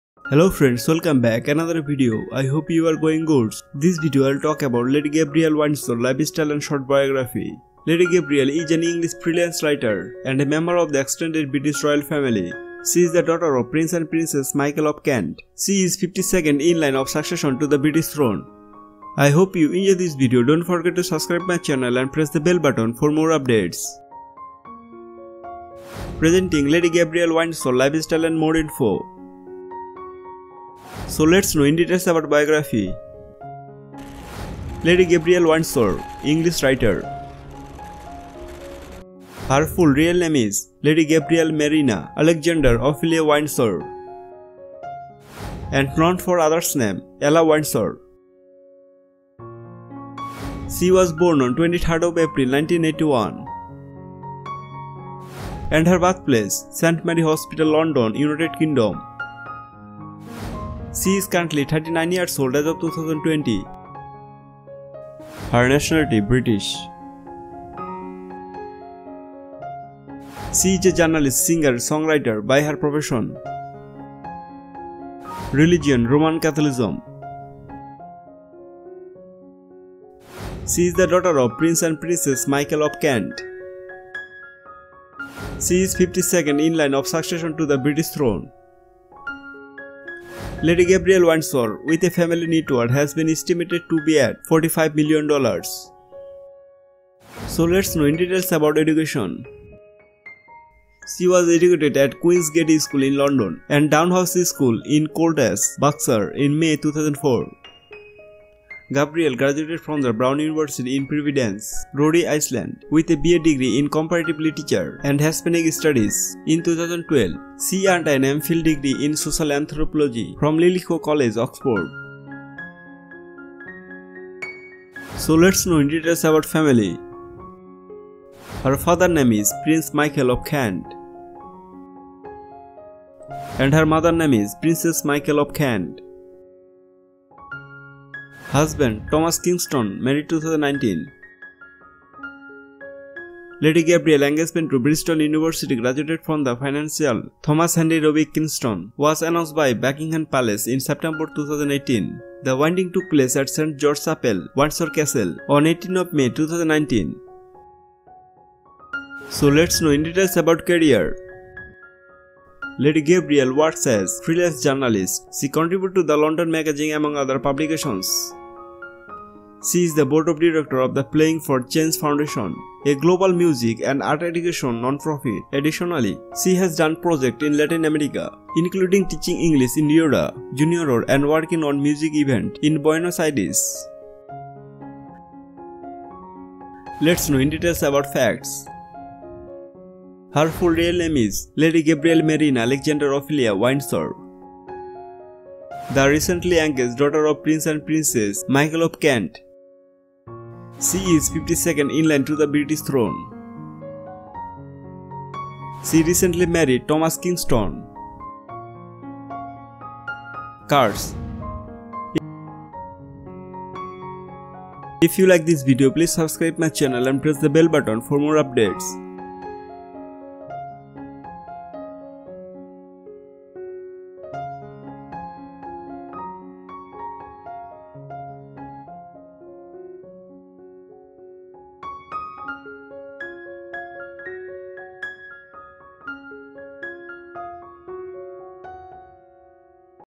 Lady Gabriella Windsor of England Lifestyle || Bio★Family★Age★Education★Facts★Career & More Info
Hello Welcome back, I hope you are going goods. Today we are taking about Lady Gabriella Windsor Lifestyle and short Biography...Lady Gabriella  is an English freelance writer and a member of the extended British royal family. She is the daughter of Prince and Princess Michael of Kent. She is 52nd in the line of succession to the British throne.